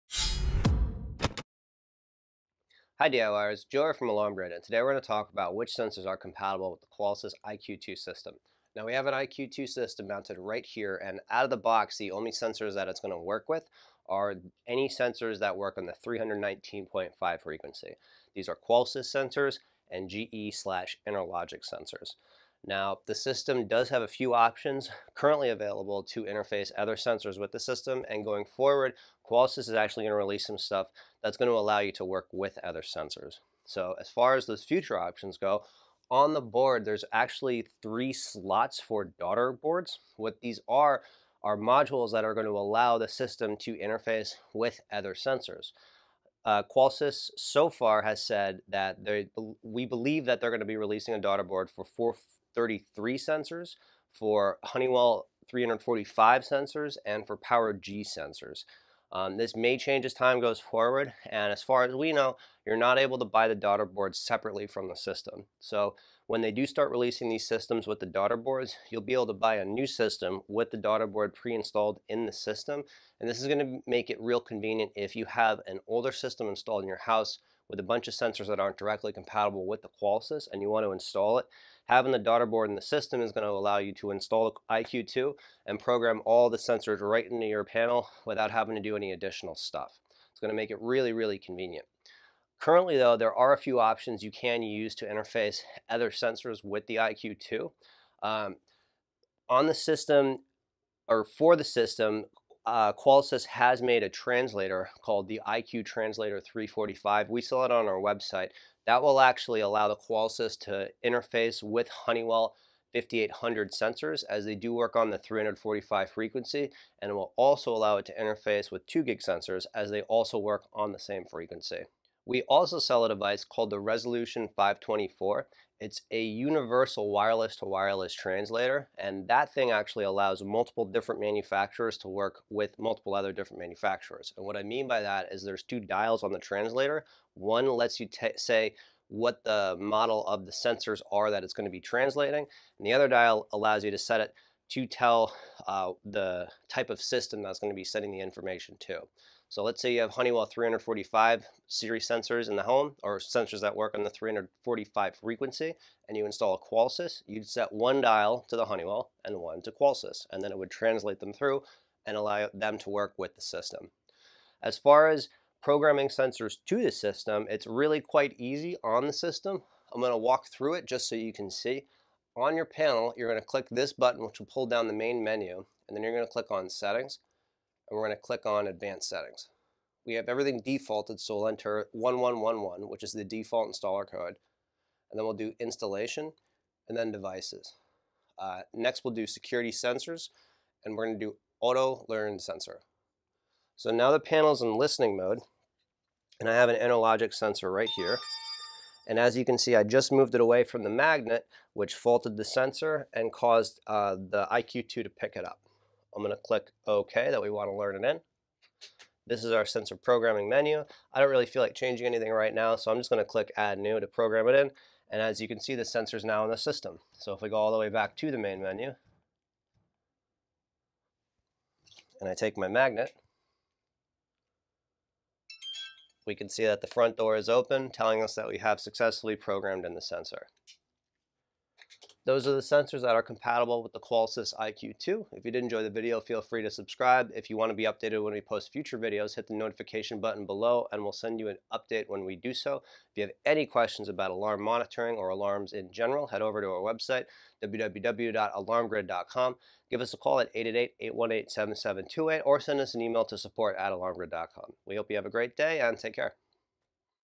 Sensors Compatible with a Qolsys IQ Panel 2 Alarm System
In this video, Joe talks about the sensors that can be used with the Qolsys IQ Panel 2. The standard IQ Panel 2 System includes a built-in wireless receiver for supporting 319.5 MHz wireless sensors. This includes sensors from Qolsys and Interlogix/GE. The system will support a maximum of 128 zones.

Setting up sensors is one of the most important parts of installing a security system. The sensors will report to the panel whenever there is any activity, such a motion or a door or window being opened. Without sensors, an alarm system will have no way of knowing that something is wrong. The Qolsys IQ Panel 2 is no exception, and sensors need to be programmed with the system in order to achieve basic functionality.

The most common sensors to use with the Qolsys IQ Panel 2 are 319.5 MHz wireless sensors. These sensors can quickly and easily be learned-in with the system through programming. However, these aren't the only types of sensors that can be used with the IQ Panel 2. The panel also has two hardwired inputs for supporting normally closed hardwired sensors. Two wired sensors can connect with these inputs to be used with the system.

If a user wants to use more than two hardwired sensors, they can do so using a wired to wireless converter. The wired sensors will connect directly with the wired to wireless converter, and the converter will send a wireless signal to the panel on the behalf of the sensors. A great wired to wireless converter to do this with is the Qolsys IQ Hardwire 16-S. By using the "S-Line" version, all wireless communication will be protected using rolling code encryption.

However, the new Qolsys IQ Panel 2 Plus Systems can use other types of wireless sensors. Each IQ Panel 2 Plus contains a PowerG daughtercard for 915 MHz PowerG Sensors. Additionally, they each contain an a single "legacy" daughtercard for supporting a different set of wireless sensors. This legacy card can be one of three frequencies, these being 319.5 MHz (Qolsys & Interlogix/GE), 345 MHz (Honeywell 5800 Series and 2GIG), or 433 MHz (legacy DSC).